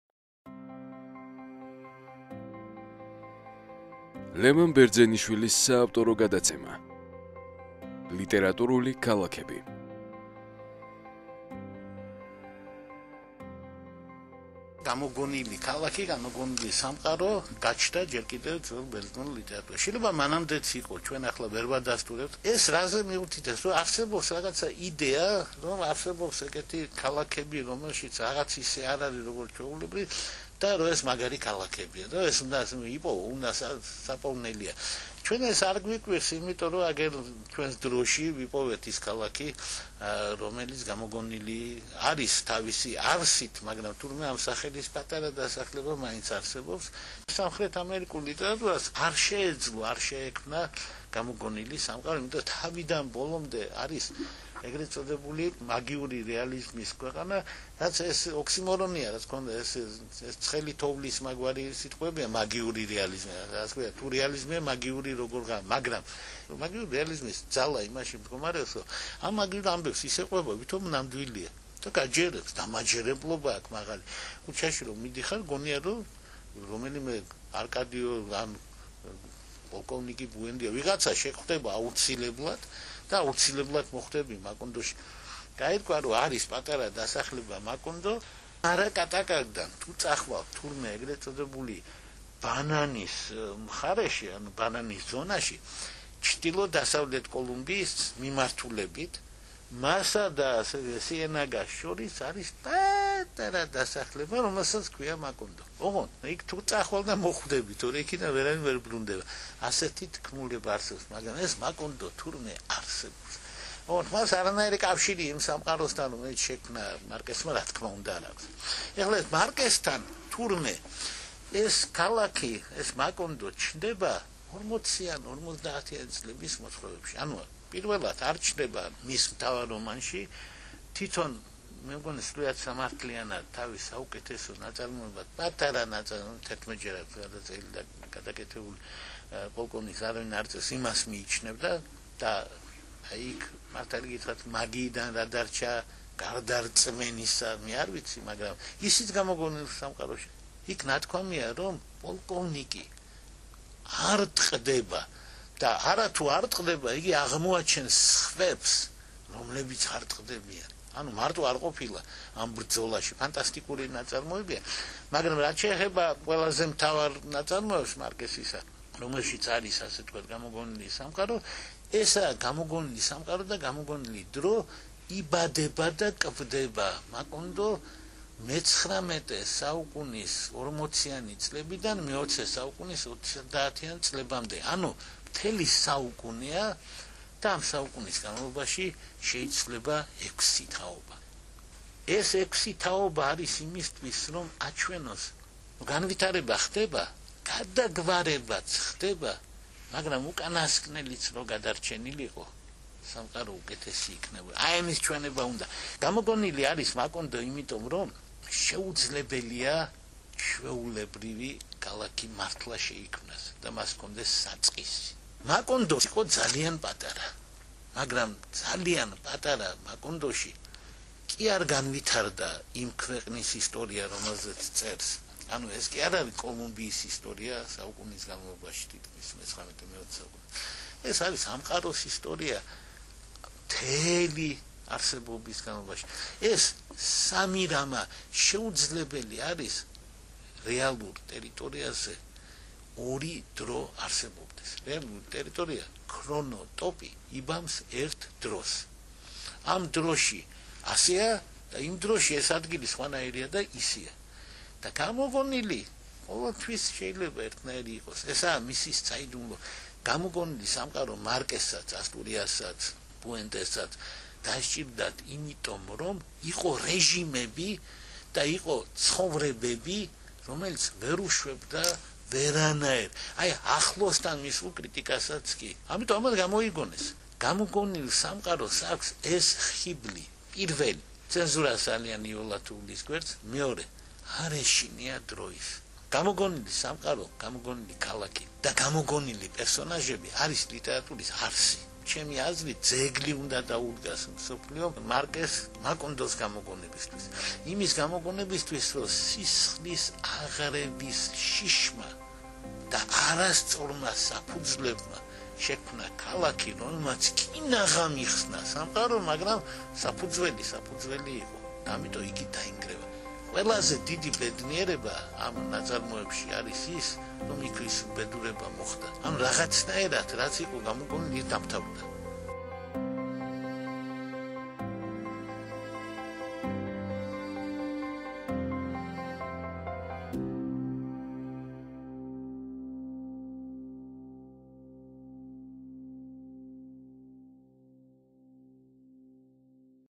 ლევან ბერძენიშვილი ► მაკონდო | „ლიტერატურული ქალაქები“
⭕️ გამომცემლობა ვანგოგენი • Vangogen Publishing წარმოგიდგენთ:

🔉ლევან ბერძენიშვილის საავტორო გადაცემას ►„ლიტერატურული ქალაქები“

🎙 გადაცემის ყოველი ახალი ჩანაწერი ვებგვერდ -ზე გამოქვეყნდება.